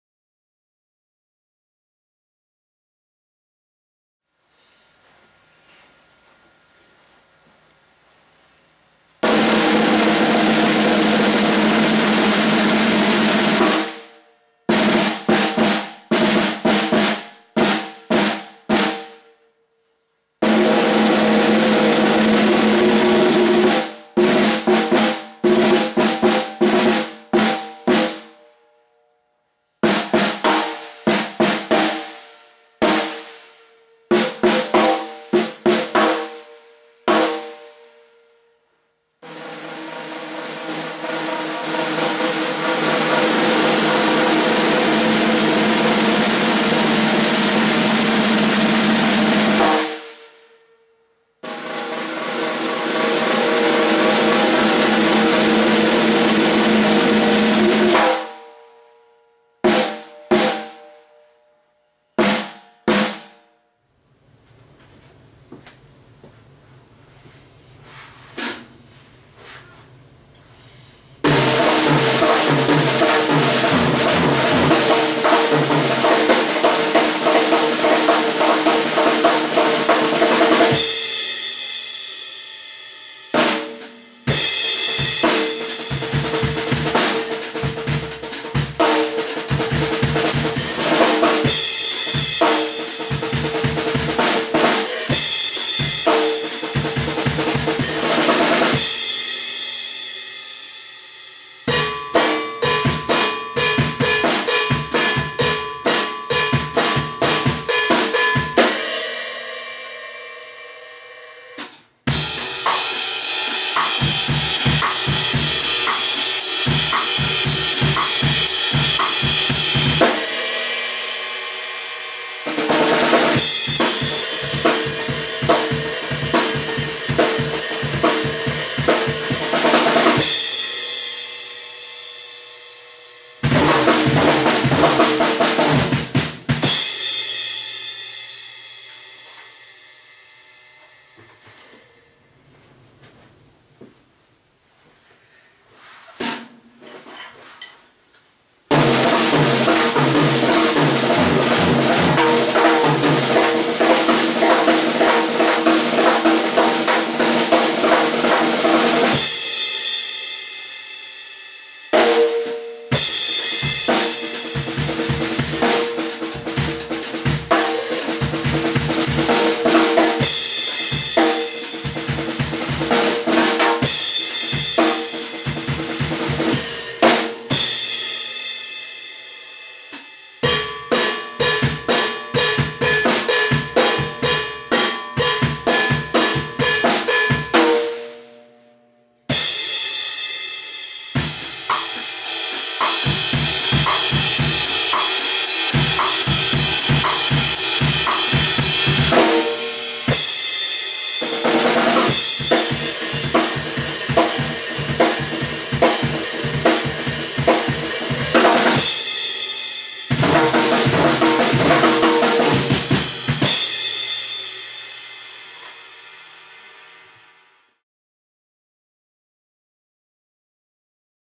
Ludwig metal snare demo. Black Magic vs. Hammered Bronze Supra
Played my two Luddy snares side by side.  Black Magic prototype (notice no badge?) has Evans Genera HD top head, the bronze has Evans Genera coated.  Both are tuned to a tension of about 90 on the drum dial.